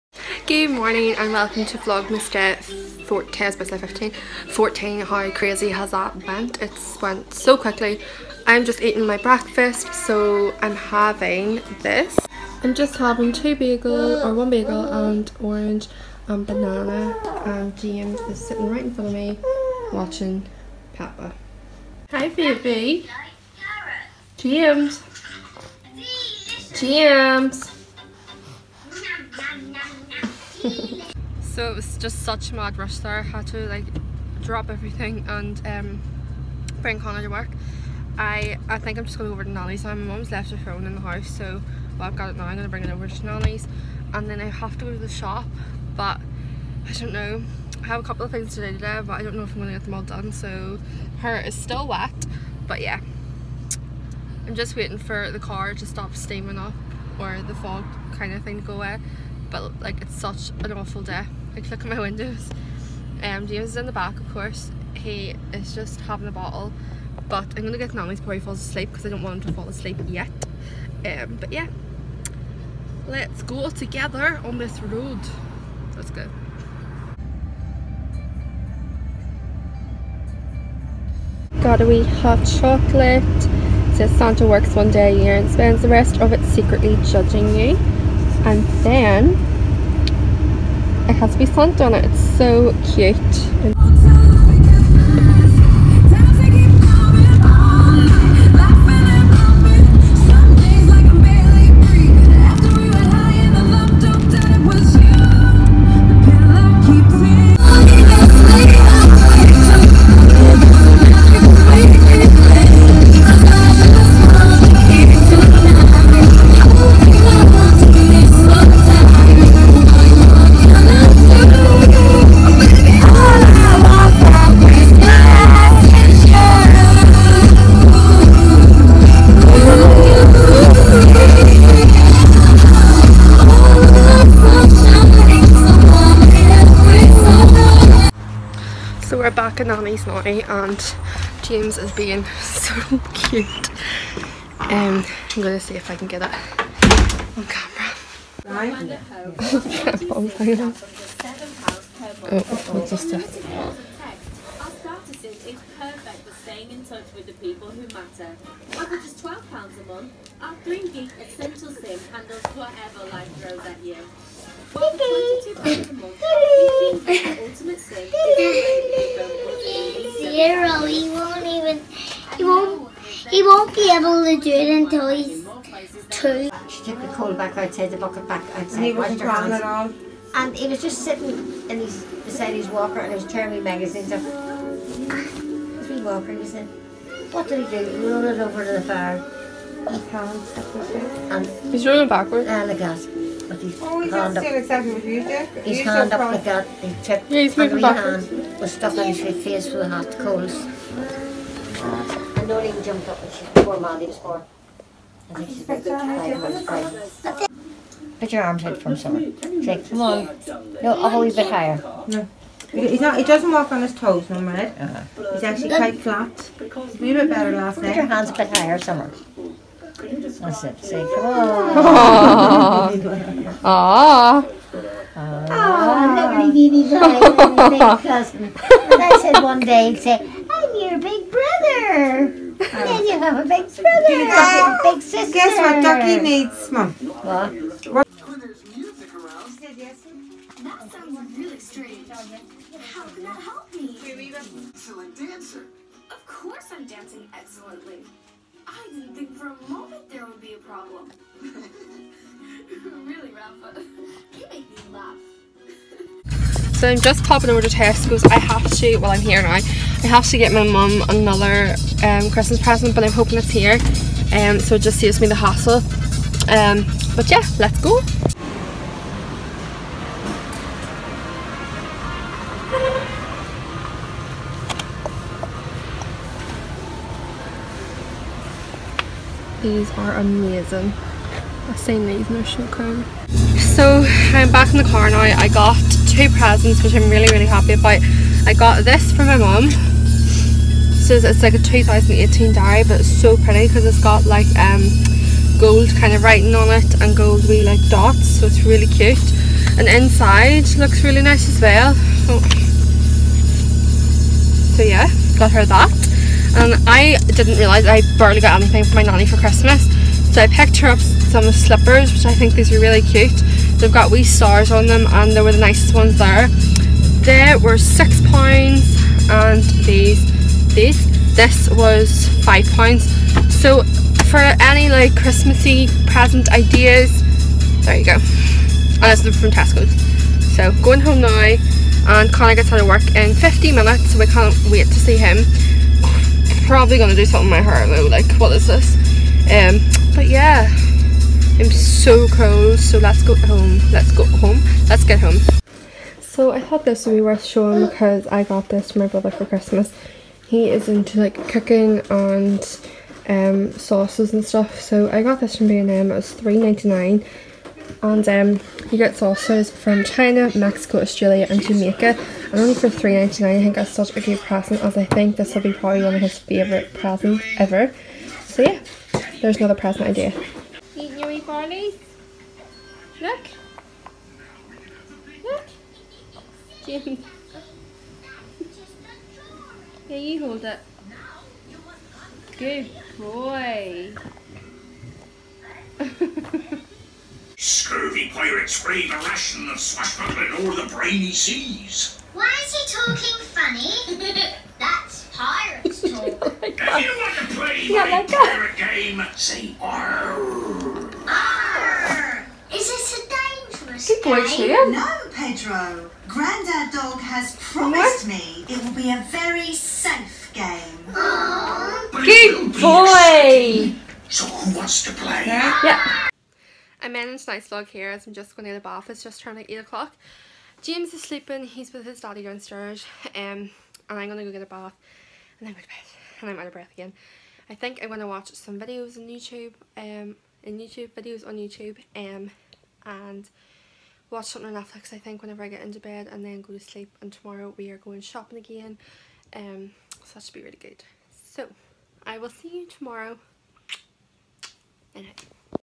FUN DAY WITH HIS COUSIN & PRESENT IDEAS! | VLOGMAS DAY 14
I hope everyone has a lovely, safe and festive Christmas! My family send lots of love to yours ❤️ xxx